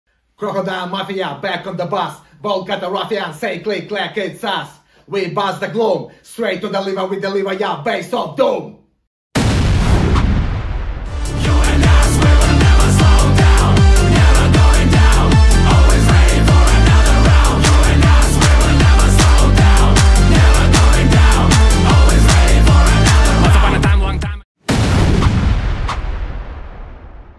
Moscow Death Brigade - Tour November 2024 Invite
BASS OF DOOM Tour 2024
9/11 LORIENT (F) PitDog
10/11 Clermont-Ferrand (F) Le Lieu-dit (JUST ADDED)
11.11 PARIS (F) GLAZART
12/11 NANTES (F) Ferrailleur
14/11 ANDERLECHT (B) Magasin 4
15/11 UTRECHT (NL) - DBS’
16/11 ESSEN (DE) - Don't Panic
17/11 INNSBRUCK (AU) - Live Stage
18/11 BARBERAZ (F) - Le Brin de Zinc
19/11 RORSCHACH (CH) - Treppenhaus
20/11 AMBERIEU EN BUGEY (F) Les Triplettes social Club
22/11 MONTPELLIER (F) - Secret Place
23/11 ZARAGOZA (SP)  - Sala Lo Intento
24/11 BADALONA (SP) - Estraperlo Club
25/11 MADRID (SP) - Gruta 77